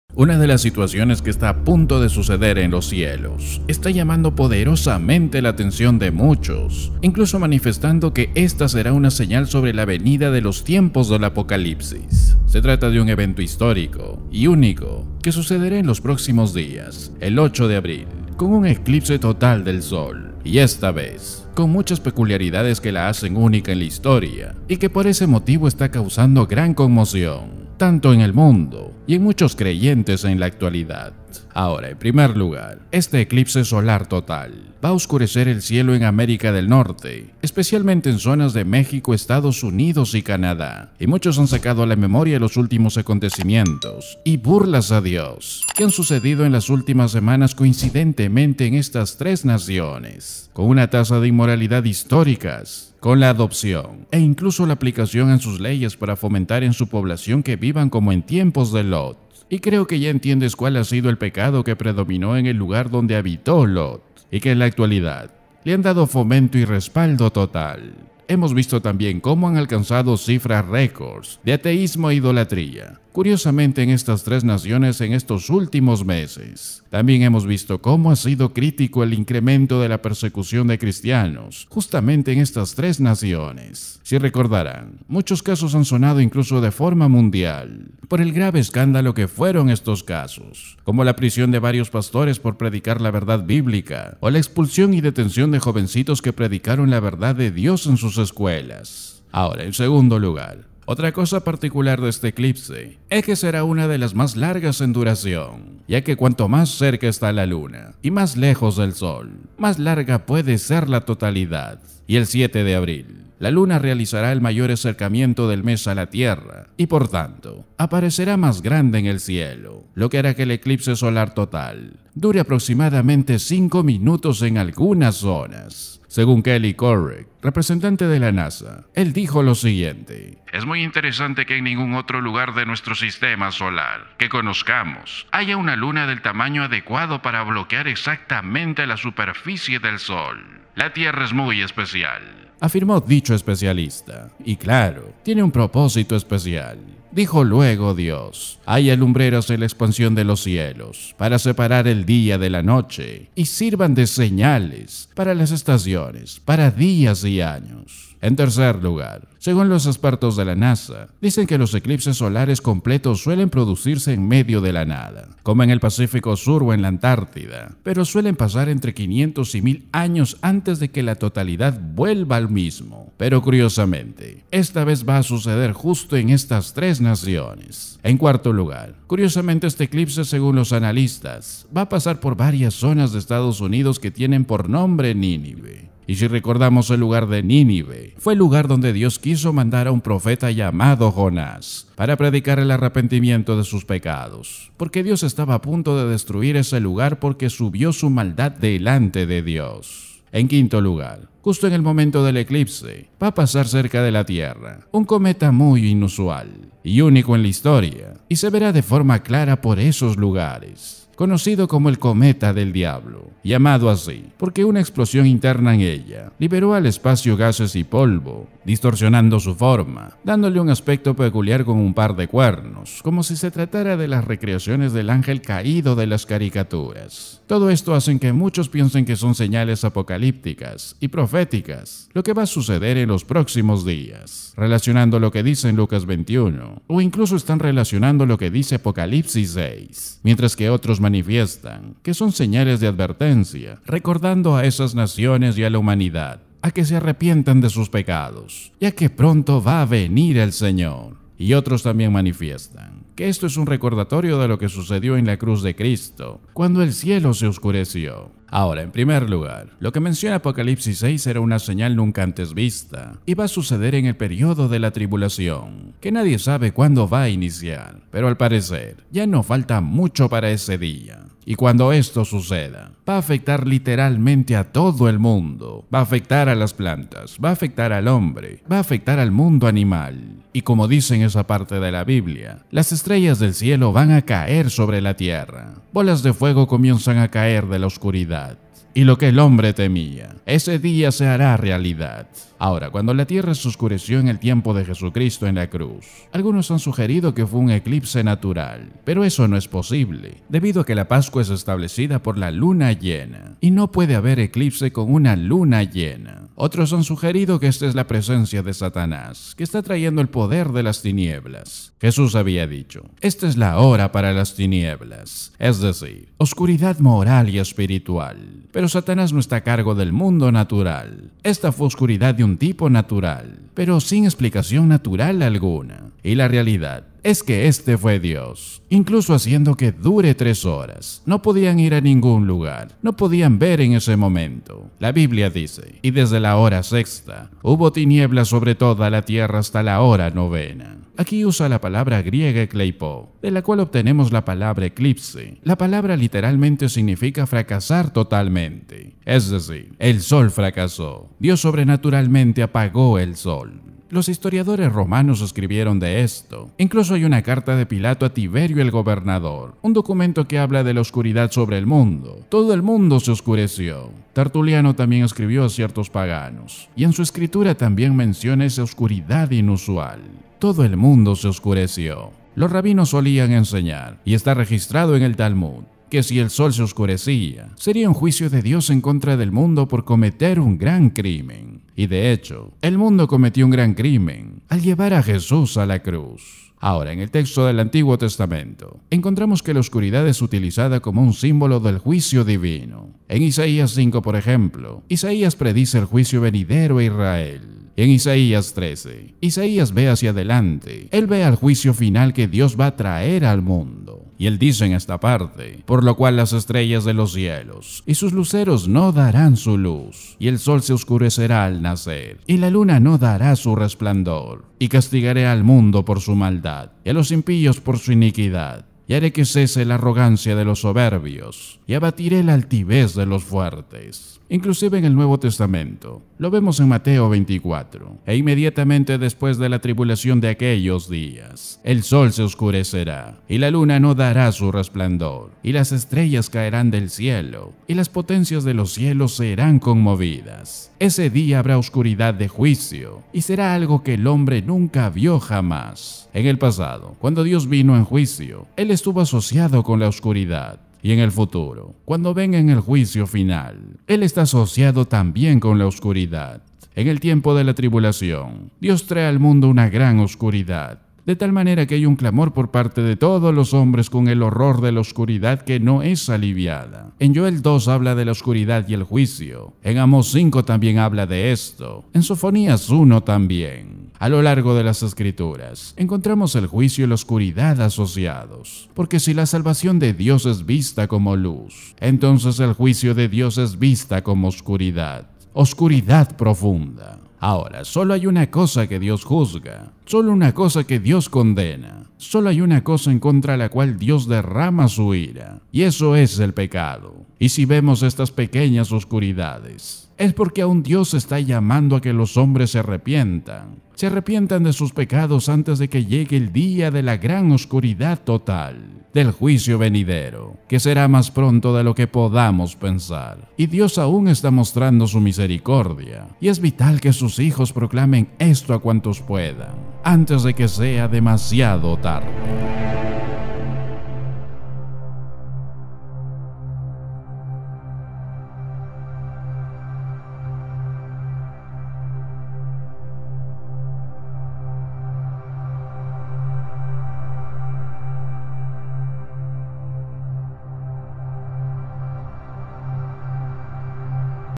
5 Datos escalofriantes sobre del próximo Eclise Solar Total... LA BIBLIA lo explica 😱📕
El gran eclipse solar total y la relación con el apocalipsis, la gente está entrando en panico por lo que sucederá el 8 de abril luego de que Norteamerica se burle de Dios,  ¿Justicia de Dios? John Macarthur y Voddie Baucham, solo la Biblia lo explica. Mira el video

ℹ️ Para más información sobre nosotros, visite nuestra página web